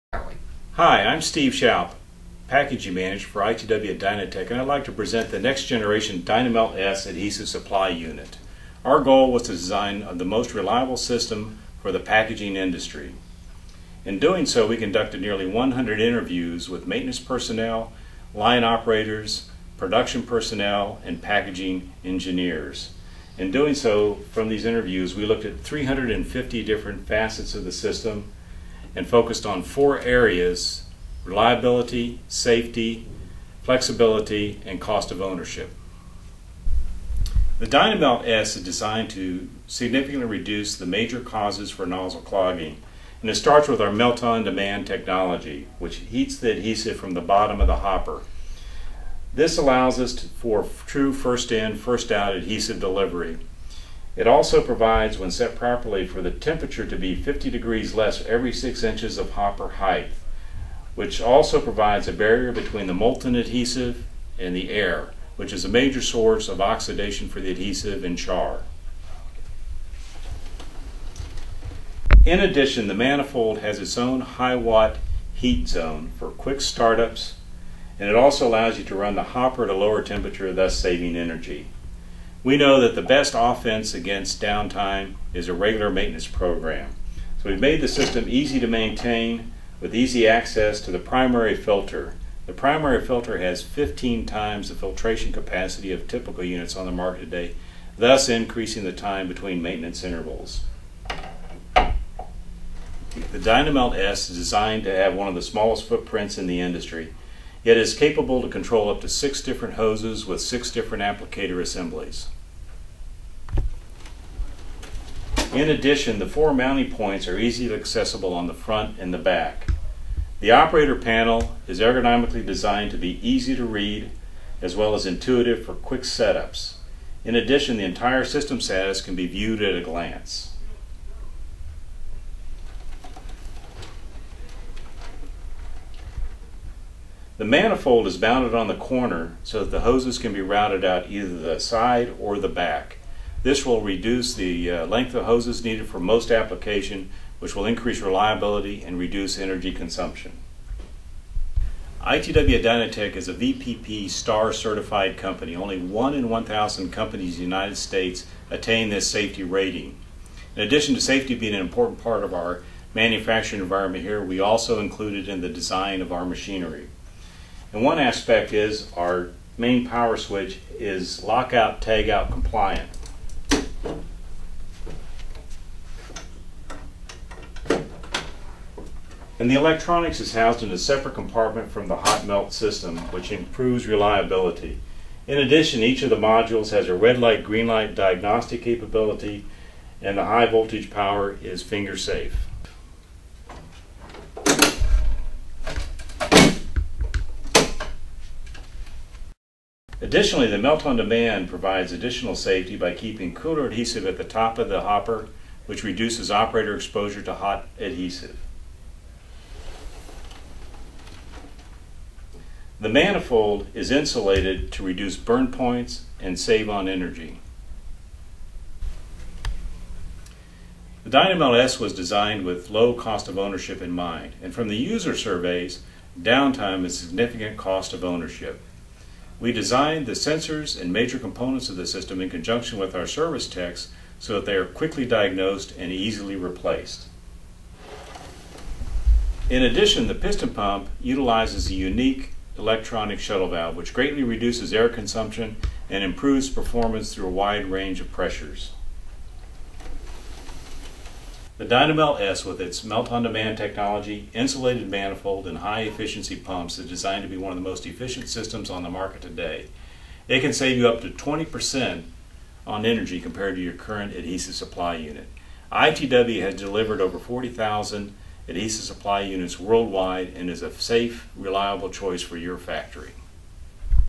Next Generation Dynamelt S Adhesive Supply Unit - The New Standard in Hot Melt Packaging
The next generation Dynamelt™ S defines a new standard of innovation in the packaging and product assembly industries. This hot melt adhesive supply unit has intuitive controls and a modular design that make it simple to install, operate and maintain, and it is the go-to choice for manufacturers worldwide. Unique design and performance features set it apart from the competition, minimizing downtime and offering greater flexibility. Designed to be used in conjunction with the Dynatec family of equipment for maximum efficiency and performance, this virtually "invisible" system will remain in sight but out of mind in your facility.